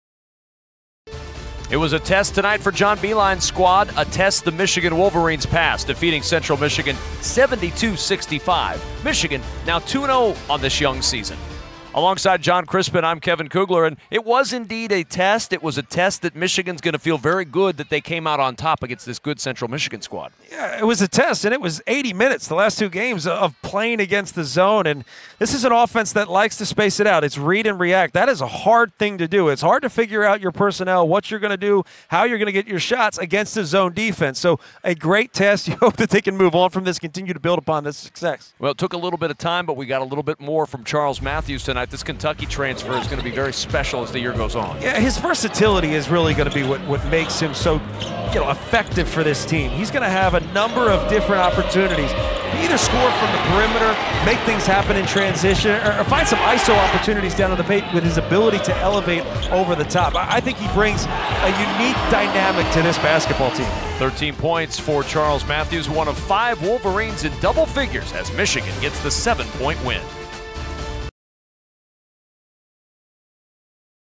Central Michigan at Michigan - Men's Basketball Highlights
Muhammad-Ali Abdur-Rahkman scored 17 points to help lead Michigan to a 72-65 win over Central Michigan.